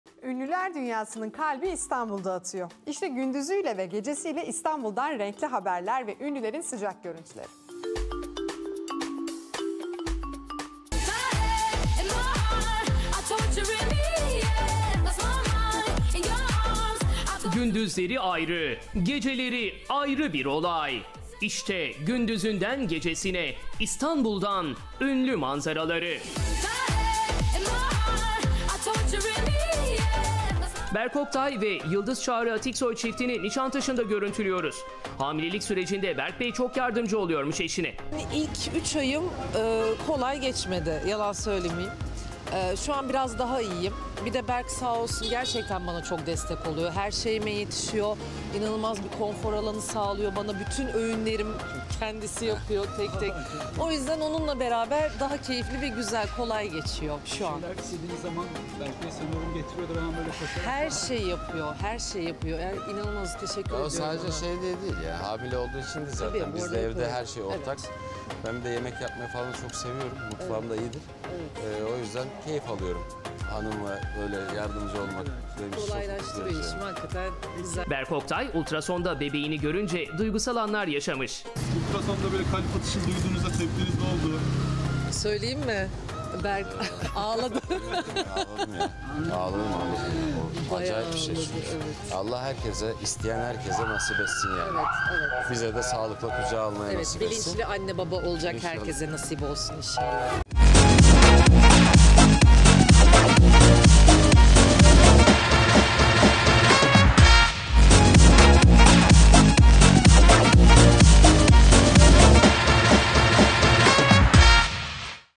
Berk Oktay ultrasonda bebeğini görünce bakın neler hissetmiş...
Nişantaşı'nda görüntülediğimiz Berk Oktay ve Yıldız Çağrı Atiksoy çiftiyle hamilelik süreci hakkında konuştuk ve bakın nasıl cevaplar aldık...